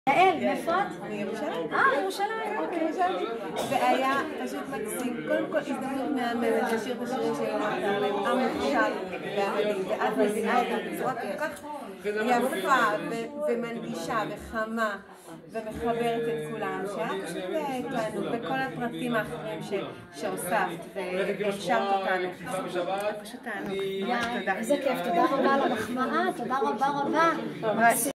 מופע טהרלב בקיסר ירושלים המלצה
מופע במלון קיסר עם ענת מקייס